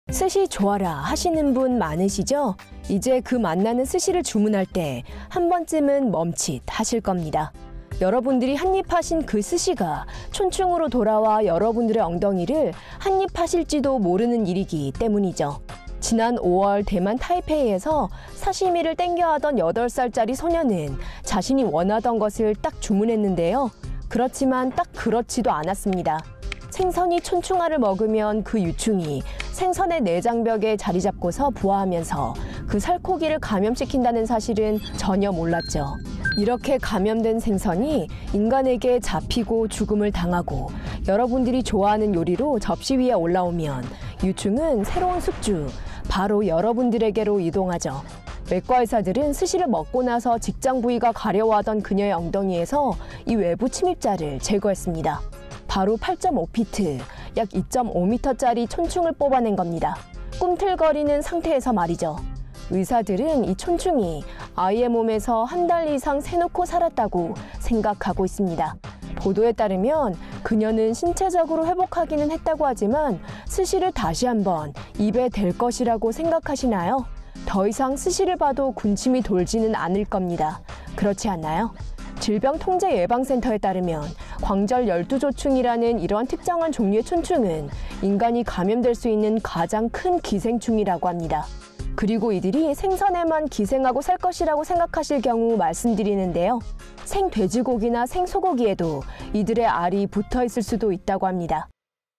8세 소녀, 스시 먹었다 엉덩이에서 2.6미터짜리 촌충 줄줄줄 뽑아내
타이페이 — 스시 좋아라하시는 분 많으시죠? 이제그 맛나는 스시를 주문할 때, 한 번 쯤은 멈칫 하실 겁니다. 여러분들이 한입 하신 그 스시가, 촌충으로 돌아와 여러분의 엉덩이를 한 입하실지도 모르는 일일 것이기 때문이죠.

지난 5월 대만, 타이페이에서, 사시미를 땡겨하던 8살짜리 소녀는 자신이 원하던 것을 딱 주문했는데요, 그렇지만 딱 그렇지도 않았습니다. 생선이 촌충알을 먹으면, 그 유충이 생선의 내장 벽에 자리잡고서, 부화하면서 그 살코기를 감염시킨다는 사실은 전혀 몰랐습니다.

이렇게 감염된 생선이 인간에게 잡히고, 죽음을 당하고, 여러분들이 좋아하는 요리로 접시 위에 올라오면, 유충은 새로운 숙주, 바로 여러분들에게로 이동하죠.

외과의사들은 스시를 먹고나서 직장부위가 가려워하던 그녀의 엉덩이에서 이 외부침입자를 제거했습니다. 바로 8.5피트, 약 2.6미터짜리 촌충을 뽑아낸 겁니다. 꿈틀거리는 상태로 말이죠.

의사들은 이 촌충이 아이의 몸에서 한달이상 세놓고 살았다고 생각하고 있습니다.

보도에 따르면, 그녀는 신체적으로는 회복하기는 했지만, 스시를 다시 한번 입에 댈 것이라고 생각하시나요?

더이상 스시를 봐도 군침이 돌지는 않을 겁니다. 그렇지 않나요?

질병통제예방센터에 따르면, 광절열두조충(diphyllobothrium latum)이라는 이러한 특정한 종류의 촌충은 인간이 감염될 수 있는 가장 큰 기생충이라고 합니다.

그리고 이들이 생선에만 기생하고 살 것이라고 생각하실 경우, 말씀드리는데요, 생돼지고기나 생소고기에도 이들의 알이 붙어있을 수도 있다고 합니다.